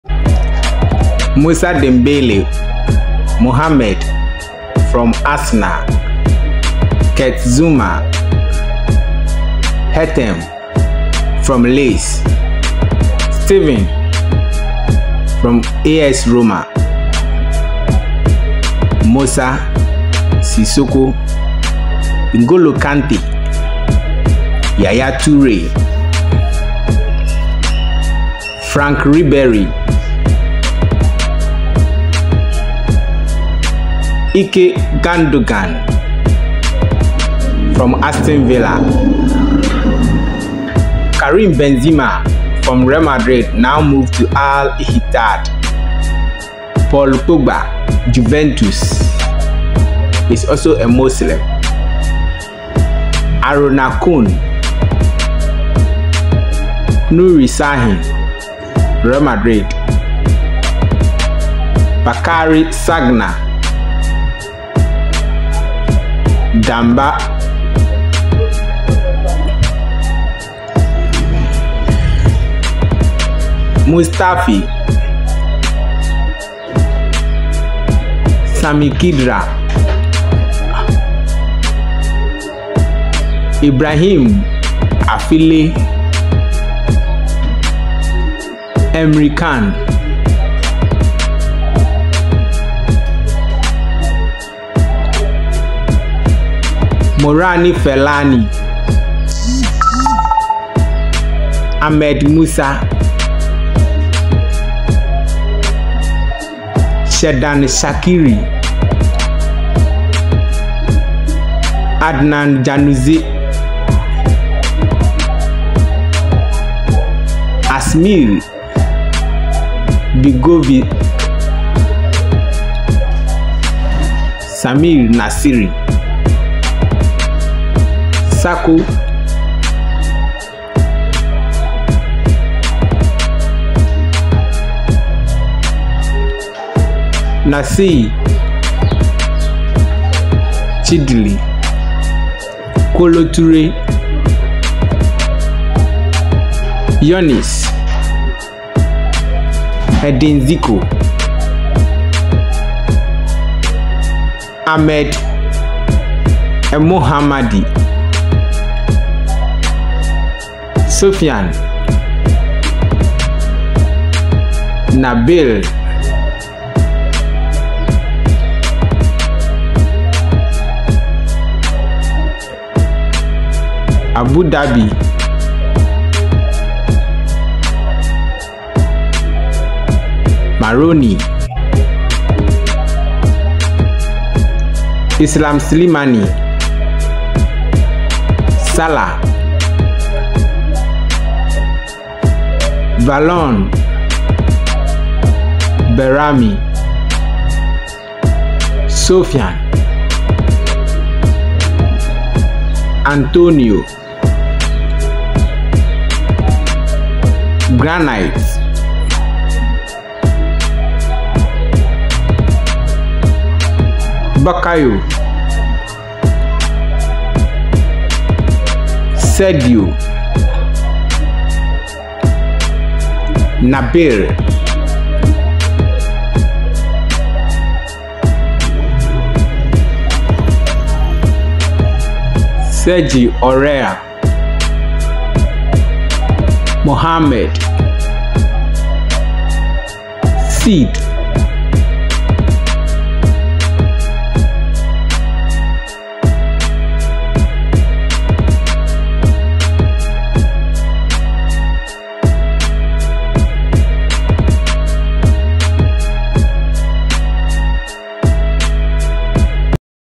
Comparison: Top 50 Muslim footballers
Football is regarded as a diverse and open sport, and there many Muslim Arsenal, Manchester United, Chelsea and Liverpool FC stars. Do you know the famous footballers on this list?